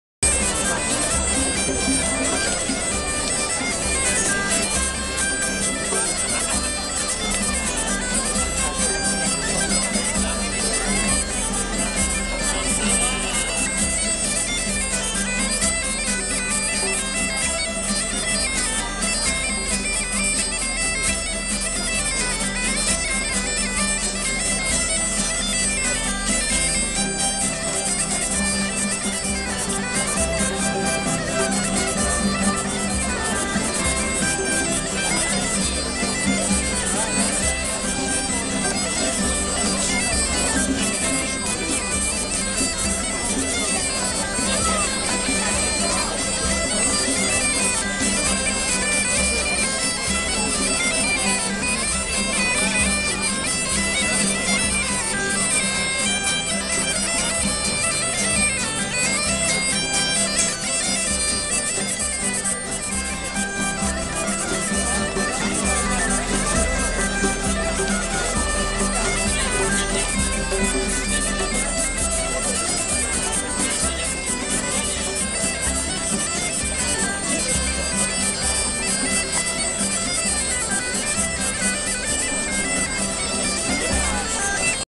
Polka Piquée - Festiv Bal Folk au Quai Saint Bernard 75005 - Août 2013
Polka piquée au FestivBal Folk du Quai Saint Bernard ) à Paris , par l'Orchestre de l'Association Quais de Seine le Jeudi 1er Août 2013 ( take 2 )